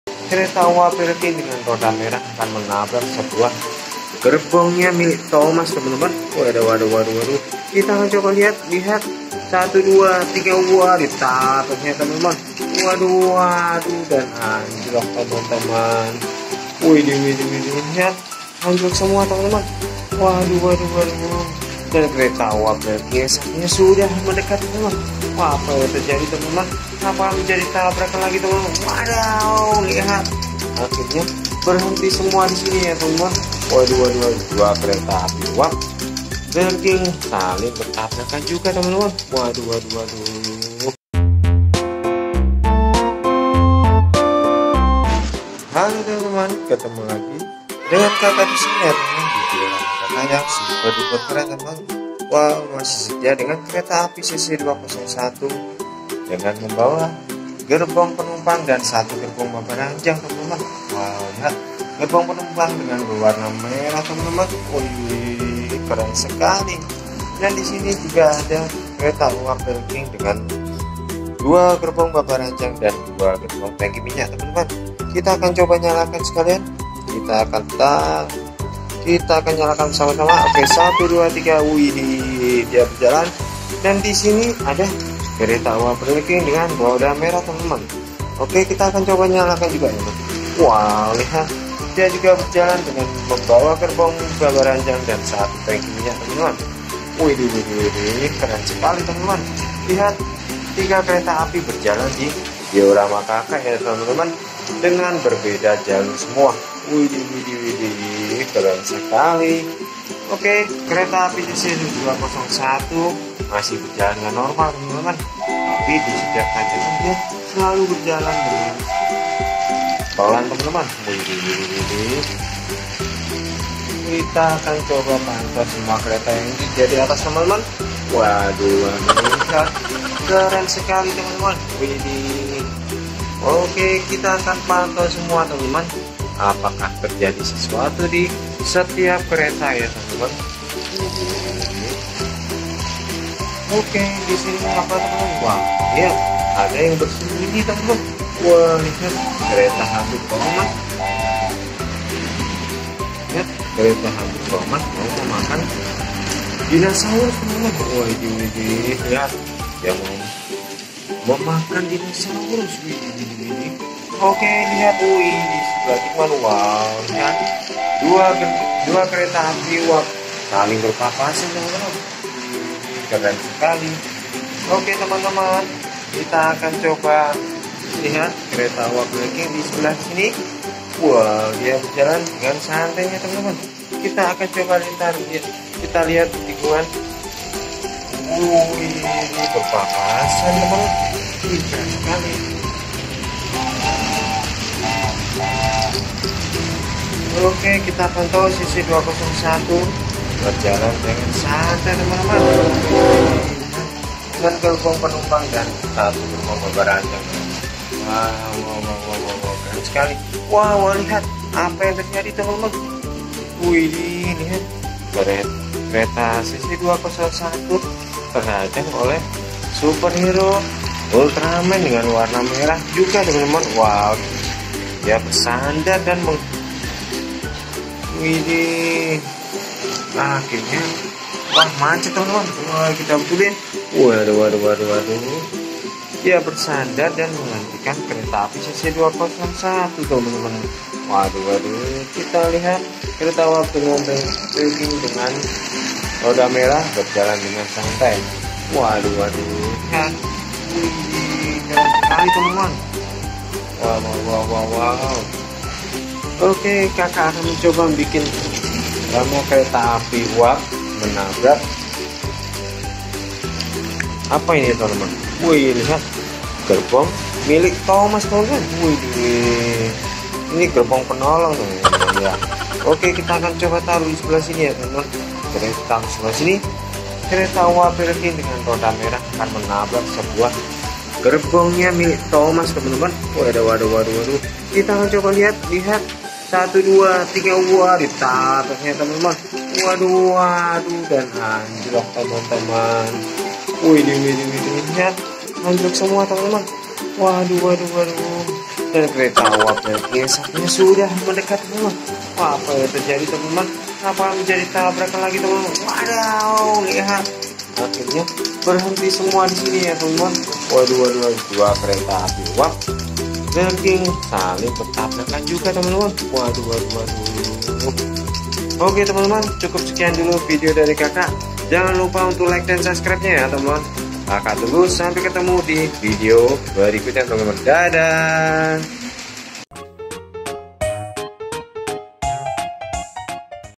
Kereta Api Mainan Miniatur Diorama Pergantian Antara Kereta Uap Dan Gerbong Thomas
jangan lupa tonton vidio yang lainnya ya teman-teman